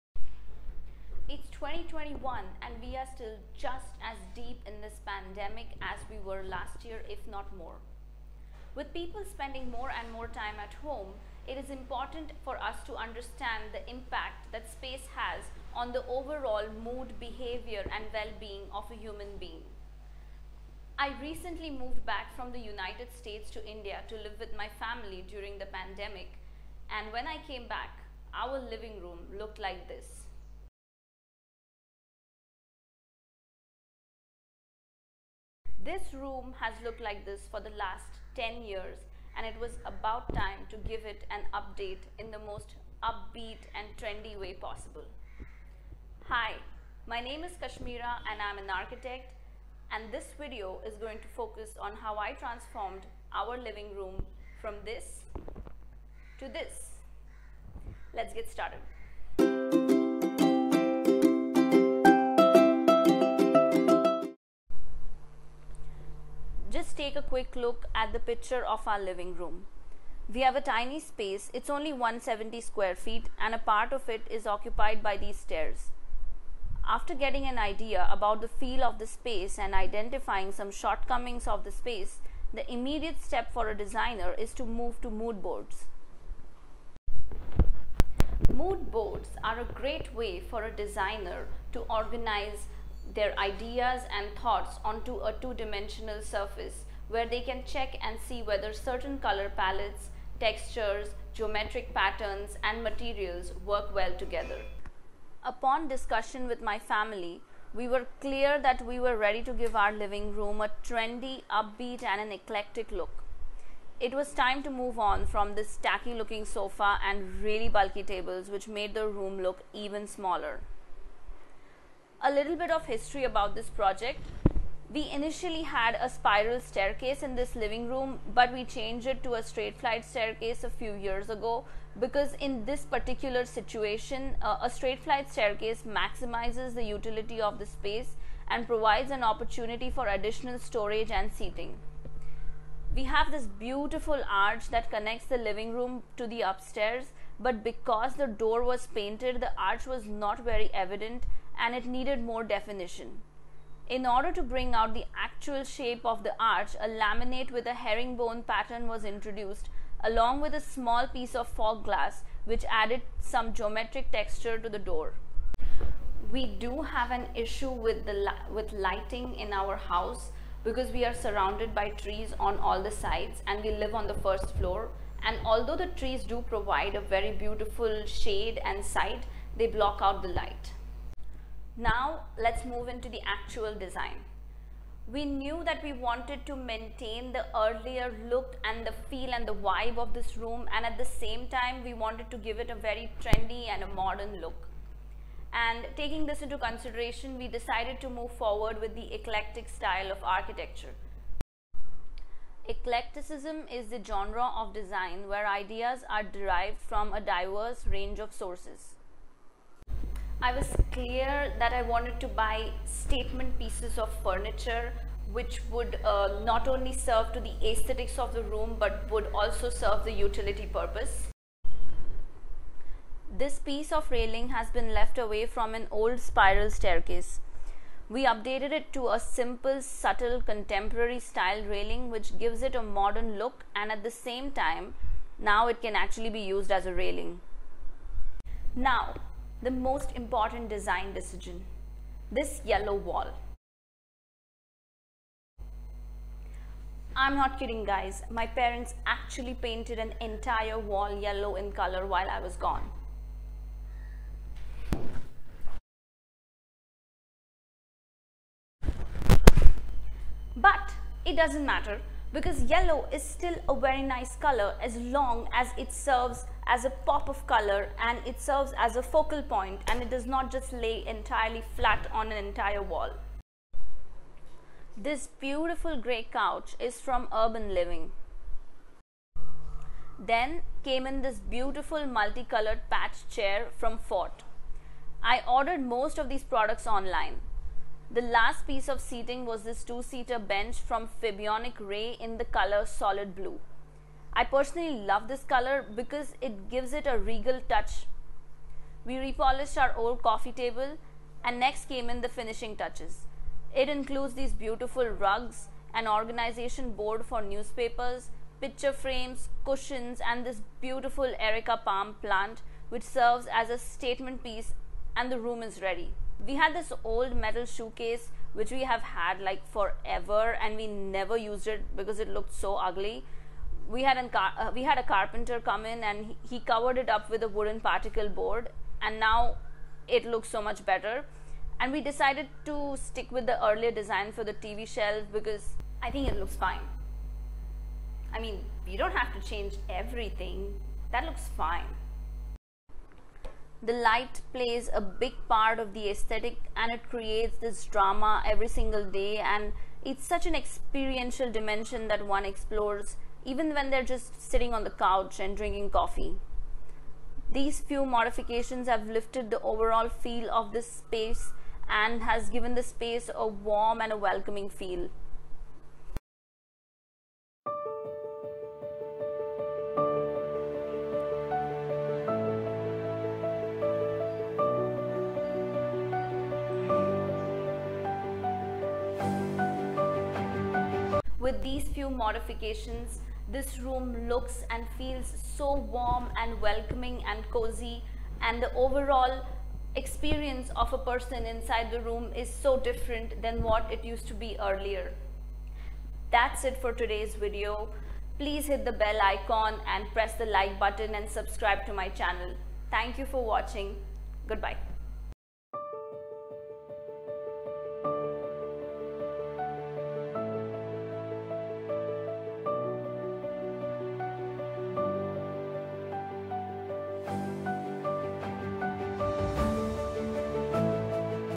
Living Room Makeover on a budget!!!
An architect transforms an abode into a cosy, comfortable and personalized space to make it more livable.
With people spending more and more time at home due to the pandemic it is important for us to understand the impact space has on human psychology, mood and overall wellbeing.